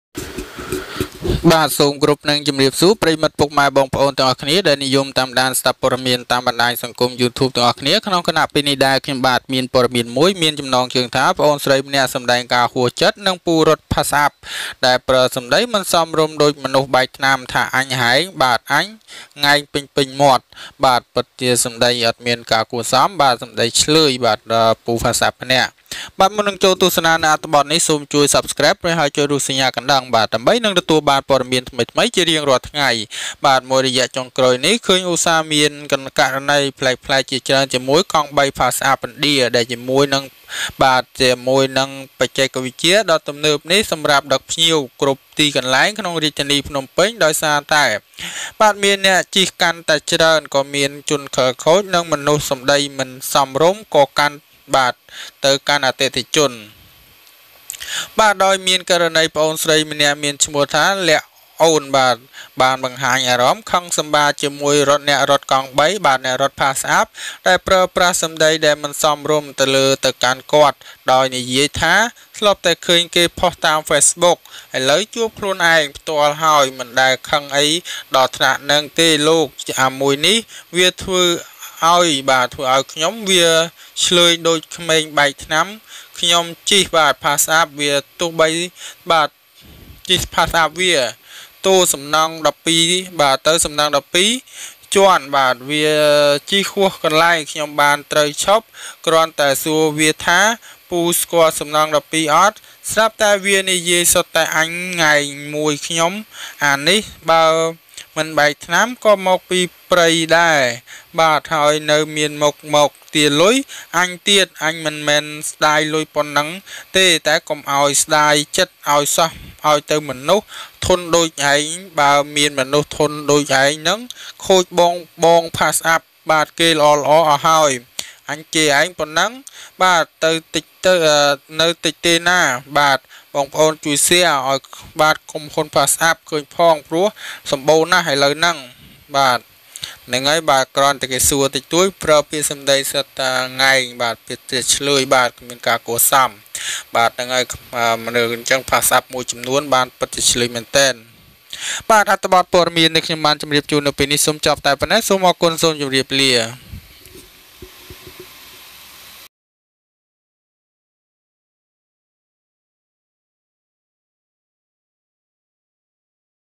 ប្អូនស្រីម្នាក់សម្តែងការហួសចិត្ត និងពូរត់ PassApp ដែលប្រើសំដីមិនសមរម្យ ដូចមនុស្សបែកថ្នាំថា អញ់ងែងពេញ
A sister expresses frustration and uncle runs PassApp with inappropriate words

សូមស្វាគមន៍មកកាន់បណ្តាញព័ត៌មានកម្ពុជាសូមកុំភ្លេចចុះឈ្មោះជាវប៉ុស្តិ៍របស់ខ្ញុំសូមអរគុណ!

សូមអរគុណយ៉ាងជ្រាវជ្រៅវ
Thank You So much !